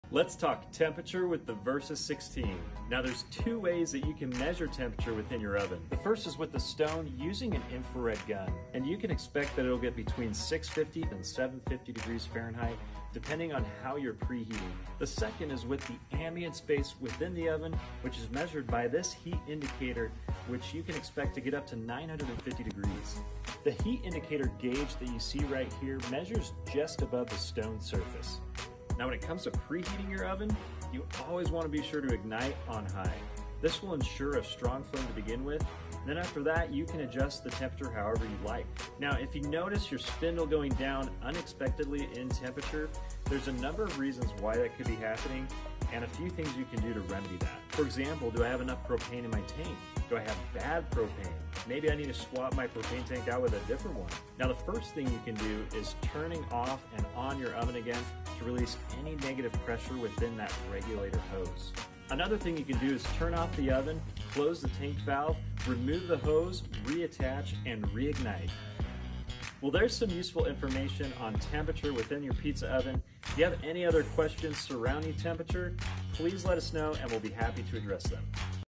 Let’s Talk Temperature with the Versa 16 Pizza Oven
The Versa’s features are designed to help you makebetterpizza with award-winning features. No longer tolerate recovery time between pizzas, doughy undercarriages, or manual turning. Rise above all that!
Key Takeaways:
-Use an infrared gun to measure stone temp
-Use the heat indicator to measure ambient temp
-Ambient temp reaches up to 950°F
-Stone temp reaches between 650-750°F
For current and future pizzaoven users, what other questions do you have about the Versa 16?